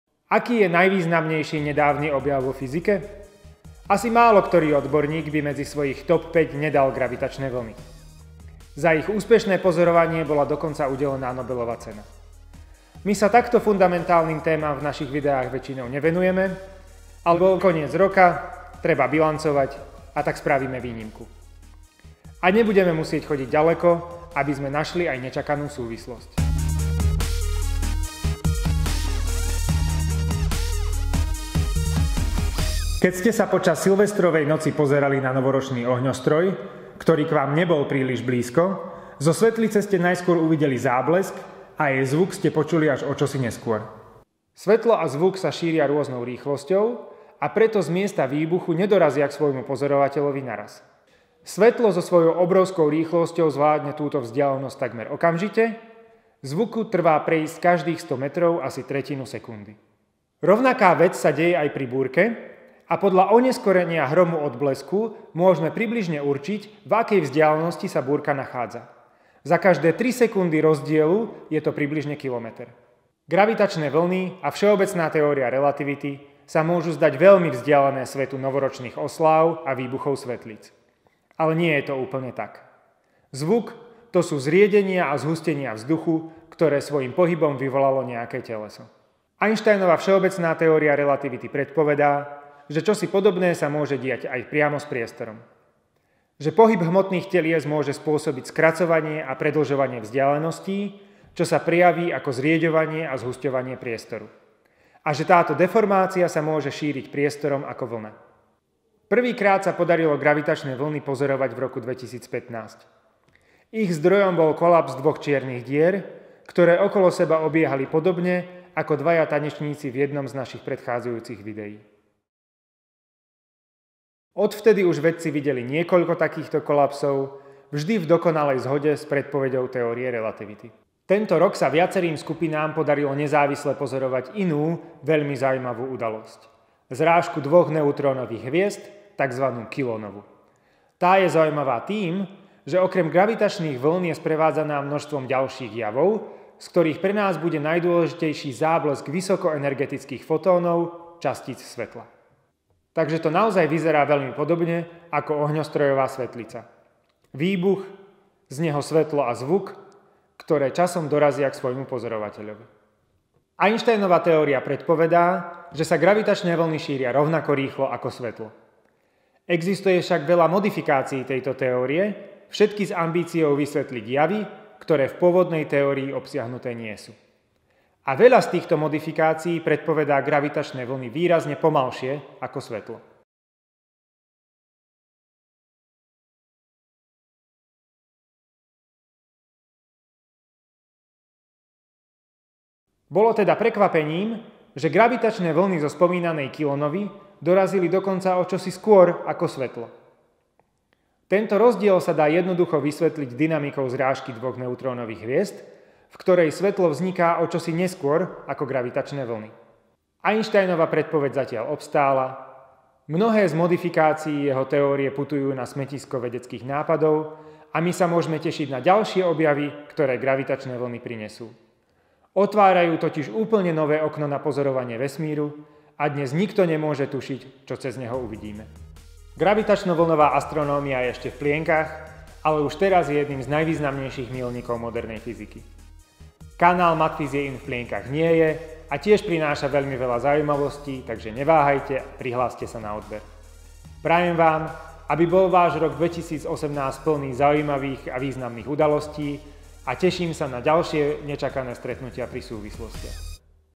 Ohňostroje a gravitačné vlny | Nečakané súvislosti
Keď ste sa počas Silvestrovskej noci pozerali na novoročný ohňostroj, ktorý k vám nebol veľmi blízko, zo svetlice ste najskôr uvideli záblesk a jej zvuk ste počuli až o čosi neskôr. Gravitačné vlny a Všeobecná teória relativity by sa mohli zdať veľmi vzdialené svetu novoročných osláv a výbuchov svetlíc. Ale nie je to úplne tak.
séria 'Nečakané súvislosti', uvádza a pripravuje Juraj Tekel:
Faculty of Mathematics, Physics, and Informatics
Comenius University, Bratislava